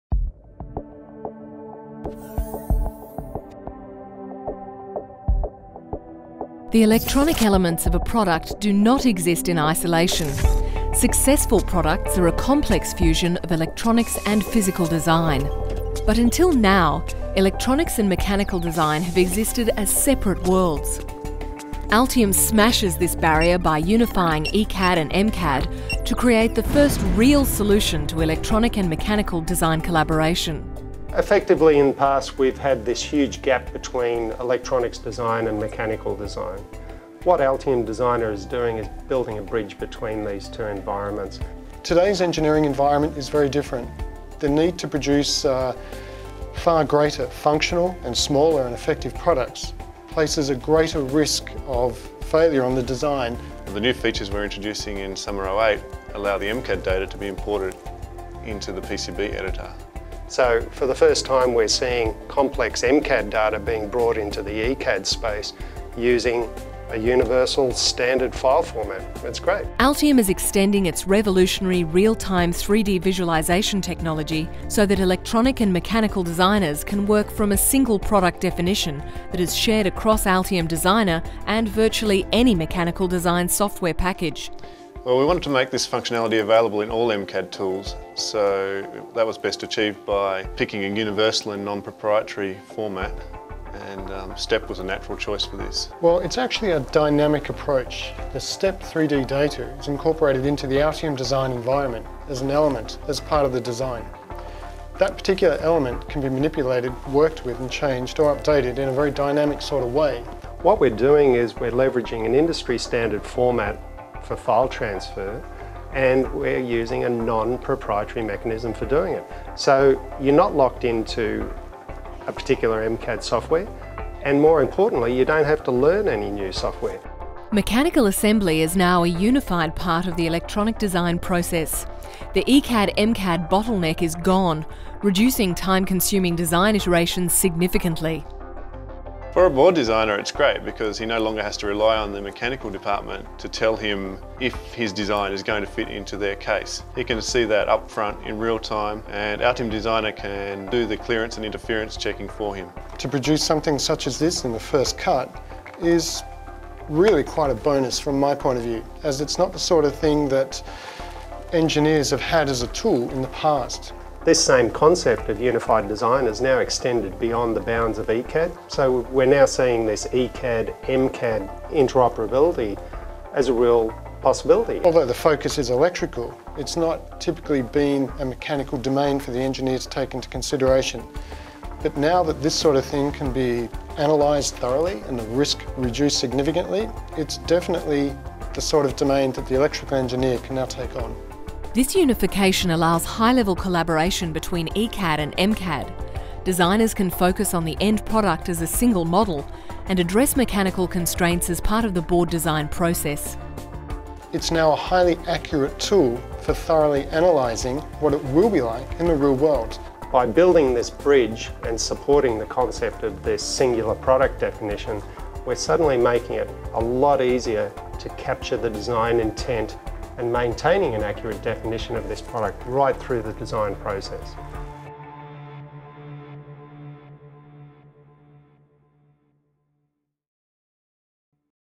Altium ECAD meets MCAD
Until now, electronics and mechanical design have existed as separate worlds. Altium smashes this barrier by unifying ECAD and MCAD to create the first real solution to electronic and mechanical design collaboration